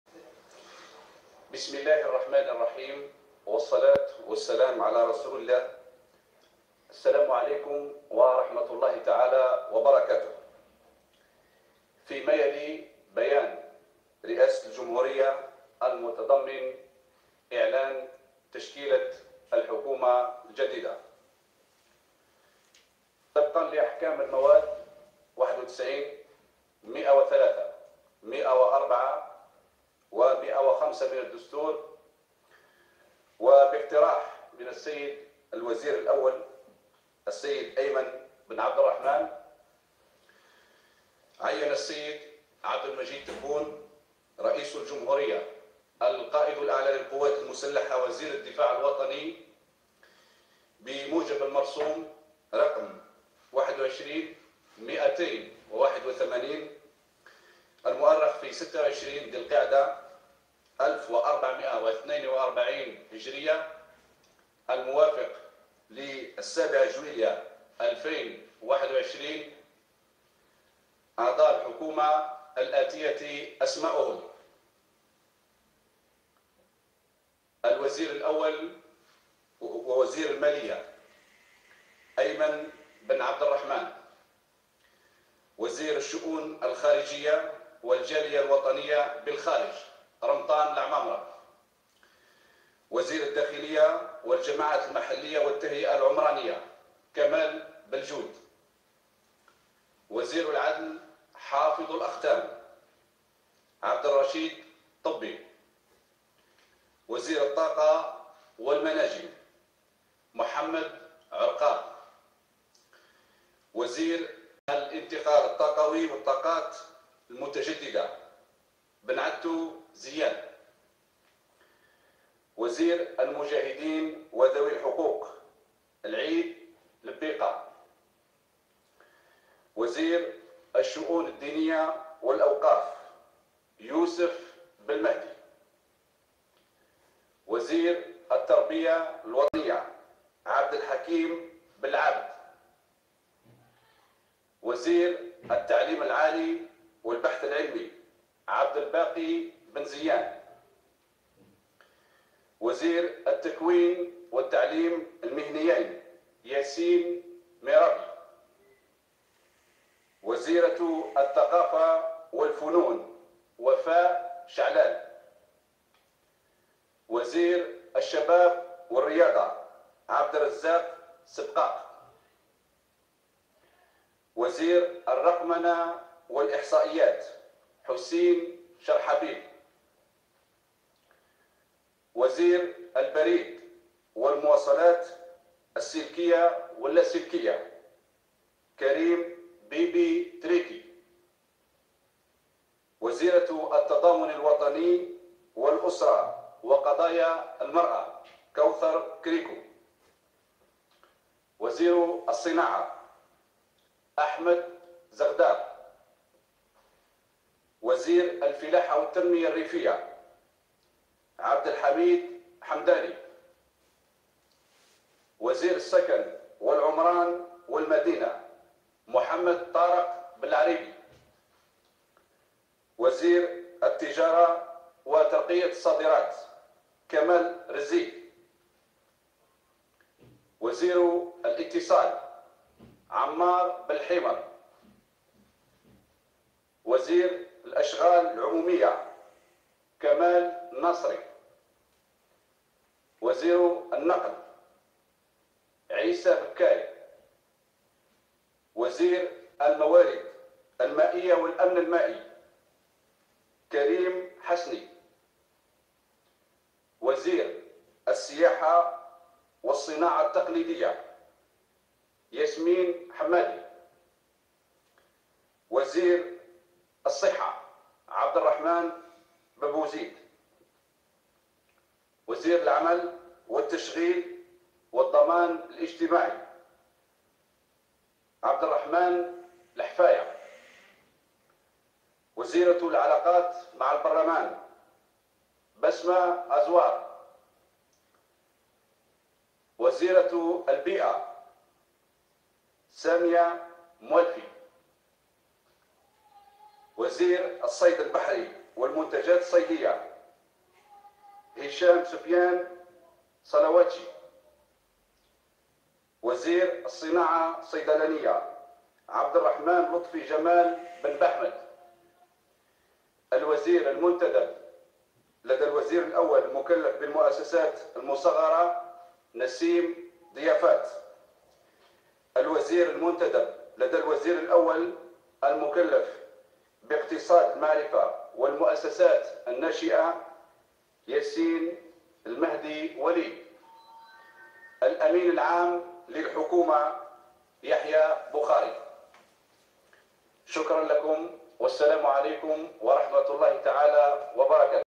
تشكيلة حكومة الوزير الأول، أيمن بن عبد الرحمان .
اعلنت رئاسة الجمهورية في بيان لها، اليوم الأربعاء، عن تشكيلة حكومة الوزير الأول، أيمن بن عبد الرحمان.

الوزير الأول ووزير المالية أيمن بن عبد الرحمان

وزير الشؤون الخارجية رمطان لعمامرة

وزير الداخلية كمال بلجود

وزير العدل عبد الرشيد طبي

وزير الطاقة والمناجم محمد عرقاب

وزير الانتقال الطاقوي والطاقات المتجددة بن عتو زيان

وزير المجاهدين وذوي الحقوق العيد دبيقة

وزير الشؤون الدينية والاوقاف يوسف بلمهدي

وزير التربية الوطنية عبد الحكيم بلعابد

وزير التعليم العالي والبحث العلمي عبد الباقي بن زيان

وزير التكوين والتعليم المهنيين ياسين مرابطي

وزيرة الثقافة والفنون وفاء شعلال

وزير الشباب والرياضة عبد الرزاق زلقان

وزير الرقمنة والاحصائات حسين شرحبيل

وزير البريد والمواصلات السلكية واللاسلكية كريم بيبي تريكي

وزيرة التضامن والاسرة وقضايا المراة كوثر كريكو

وزير الصناعة احمد زغدار

وزير الفلاحة والتنمية الريفية عبد الحميد حمداني

وزير السكن والعمران والمدينة محمد طارق بلعريبي

وزير التجارة وترقية الصادرات كمال رزيق

وزير الاتصال عمار بلحيمر

وزير الأشغال العمومية كمال ناصري

وزير النقل عيسى بكاي

وزير الموارد المائية والأمن المائي كريم حسني

وزير السياحة والصناعة ياسمين حمادي

وزير الصحة عبد الرحمان بن بوزيد

وزير العمل والتشغيل والضمان الاجتماعي عبد الرحمان لحفاية

وزيرة العلاقات مع البرلمان بسمة عزوار

وزيرة البيئة سامية موالفي

وزير الصيد البحري هشام سفيان صلواتشي

وزير الصناعة الصيدلانية بن باحمد

الوزير المنتدب المكلف بالمؤسسات المصغرة نسيم ضيافات

الوزير المنتدب المكلف باقتصاد المعرفة ياسين وليد

الامين العام للحكومة يحيى بوخاري